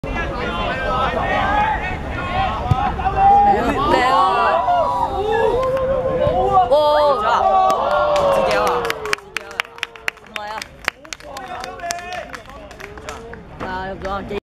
譚伯羽vs百周年(2014.12.15.屯門學界足球甲組季軍賽)之入球0:1
入球者 11號